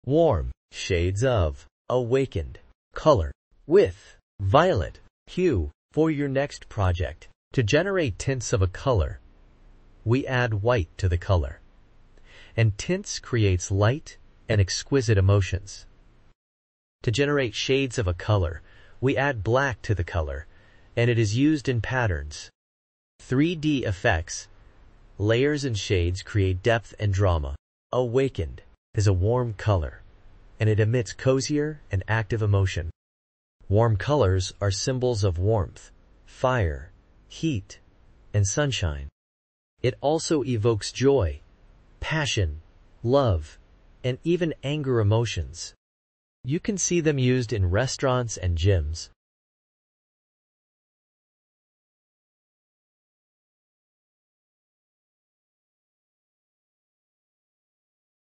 Shades & Tints of Awakened color e3dae9 A Warm Violet color
Shades of Awakened color e3dae9 Warm Shades of Awakened color with Violet hue for your project!

Shades
Hex c9c1cf

Tints
Hex e6deeb

Awakened is a warm color, and it emits cozier and active emotion. Warm colors are symbols of warmth, fire, heat, and sunshine. It also evokes joy, passion, love, and even anger emotions. You can see them used in Restaurants and Gyms.